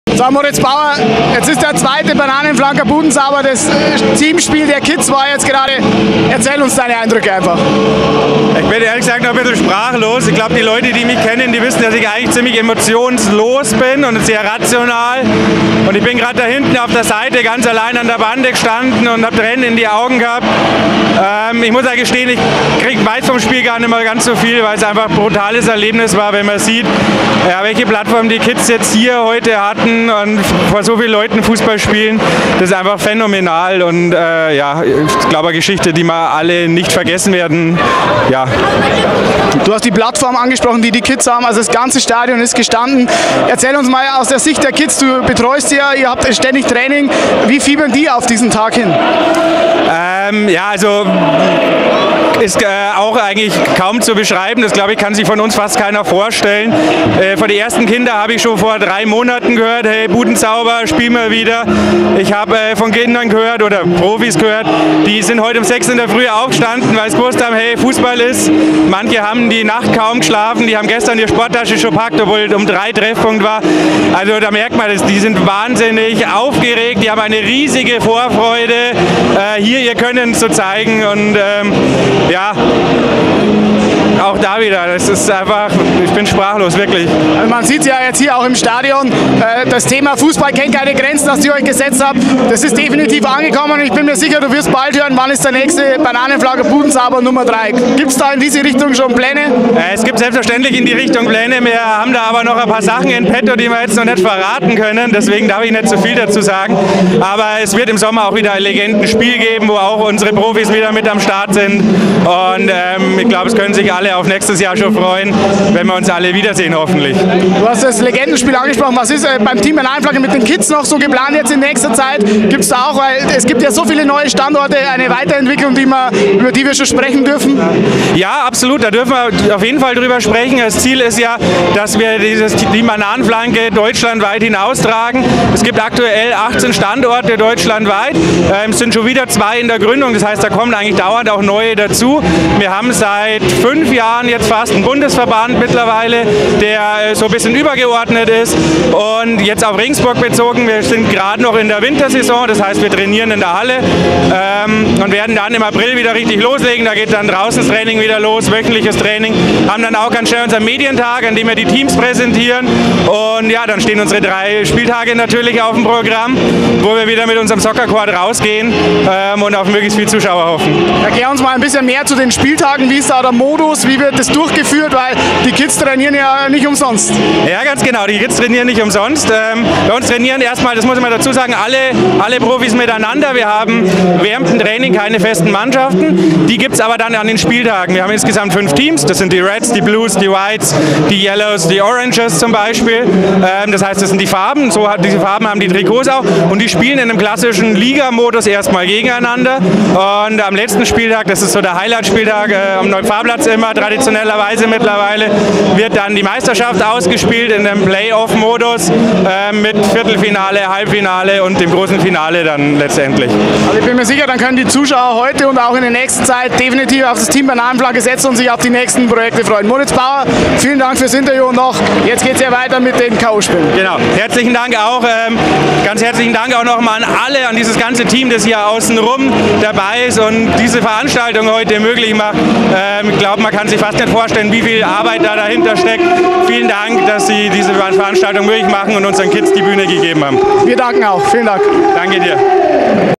Eisbären TV: Budenzauber 2020 - Interview mit Moritz Bauer
🍌🍌🍌 Mit Moritz Bauer hatte Tobias Biersack den 1. Vorstand von Team Bananenflanke Regensburg vor der Kamera. 🍌🍌🍌

Das komplette Interview seht ihr im Video. Viel Spaß.